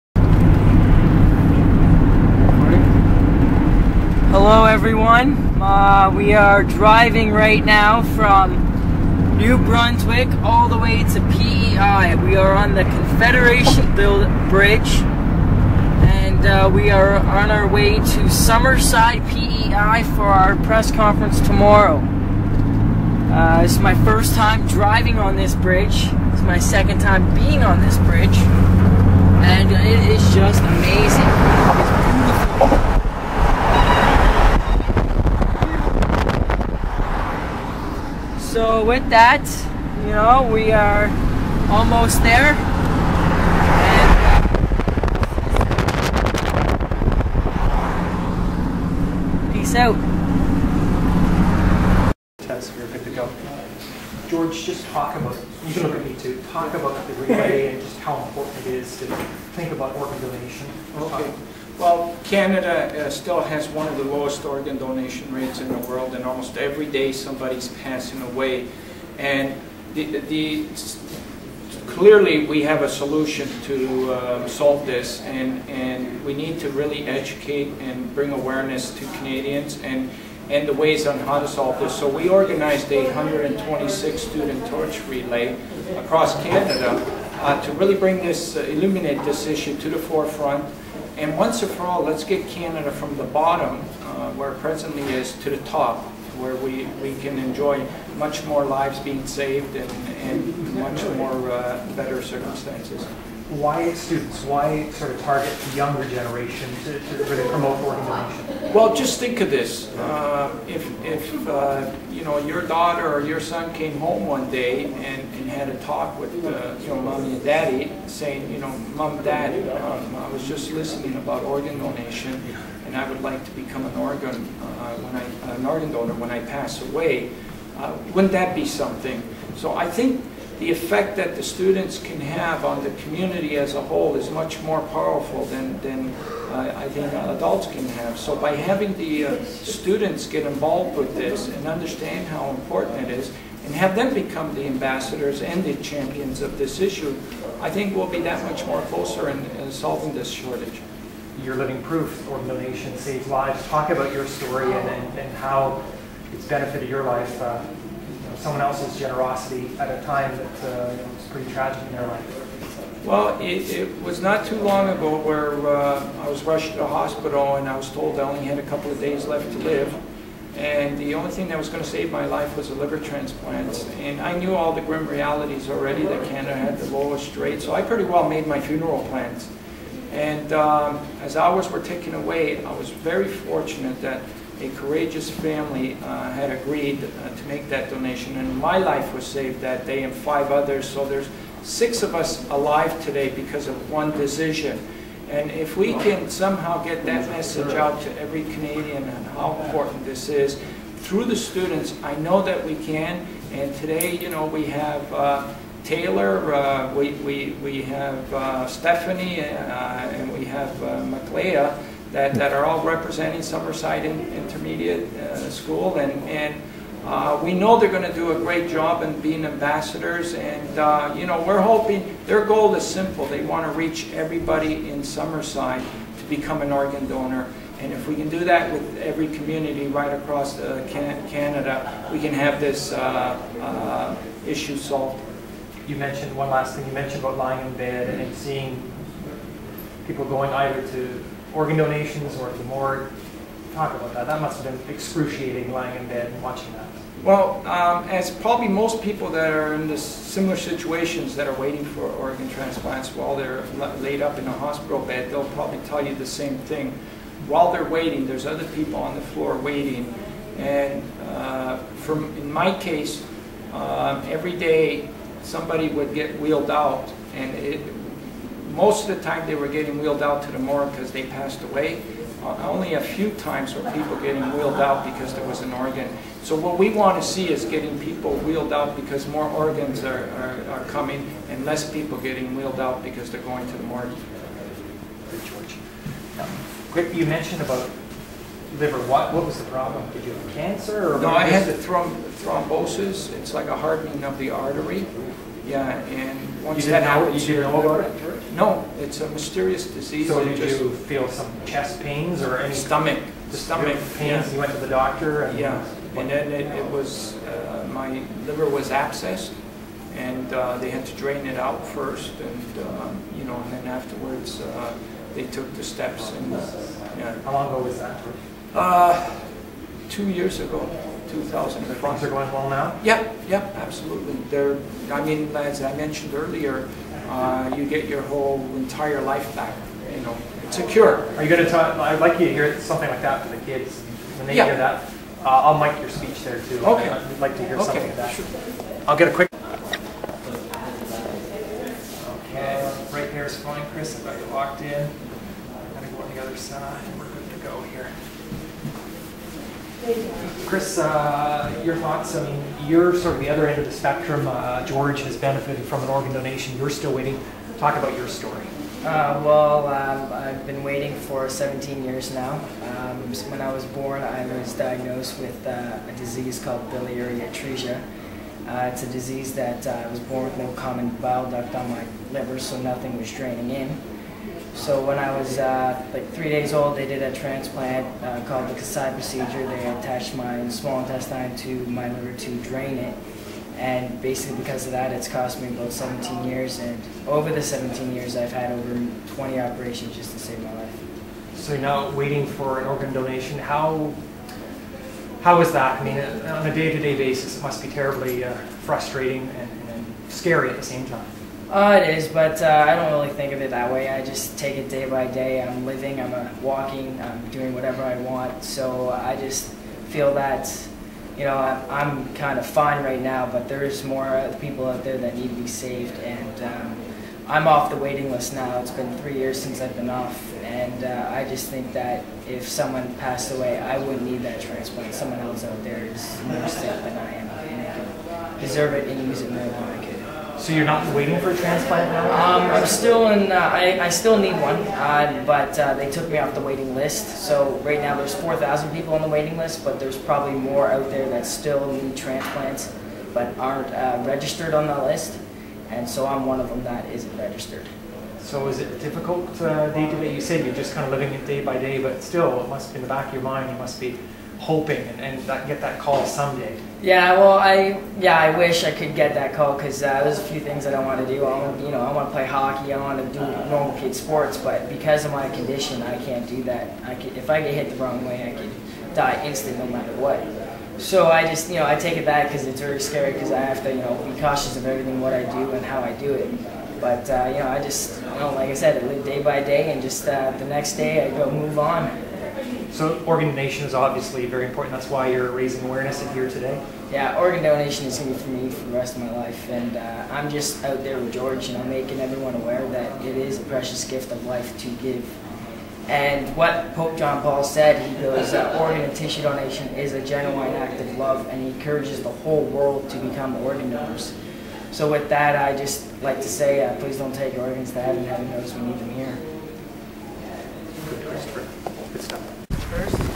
SOS4000 Summerside PE STUDENT TORCH CHAMPIONS Stephannie Ellis, Mikayla Clow & Taylor Stright. Oct 29 2007 DAY 29. CBC INTERVIEW
STUDENT TORCH CHAMPIONS Stephannie Ellis, Mikayla Clow & Taylor Stright. Over 100 Students from 100 cities/towns are relaying the Torch of Life across Canada to promote organ & tissue donations. CBC TV interviews SOS4000 Road Manager George Marcello before the Torch Trek in Summerside PE